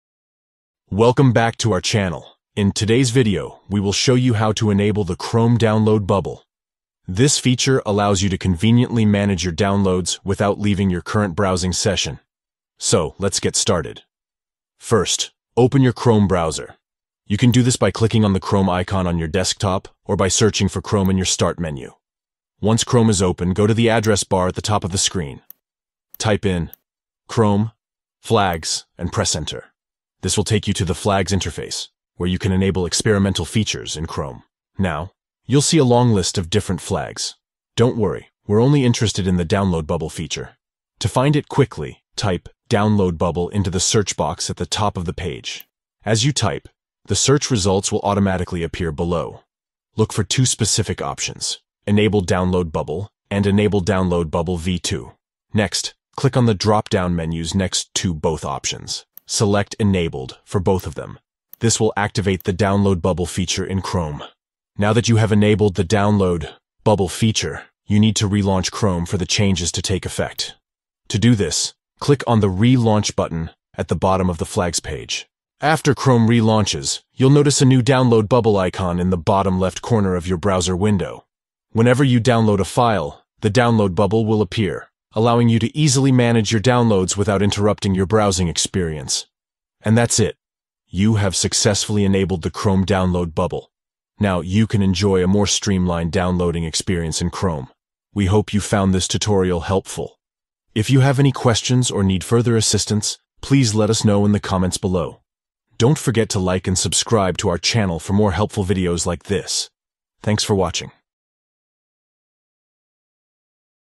How to Enable Chrome Download Bubble
In this YouTube video, we will guide you on how to enable the Chrome download bubble feature. Follow these simple steps:

1. Open Chrome.
2. Type chrome://flags in the address bar and press Enter.
3. Type 'download bubble' into the search box.
4. Select Enabled for both Enable download bubble and Enable download bubble V2.
5. Click the Relaunch button to reopen your Google Chrome.

By enabling the Chrome download bubble, you can conveniently manage your downloads without interrupting your browsing experience. Follow these steps and enjoy a more streamlined downloading experience in Chrome!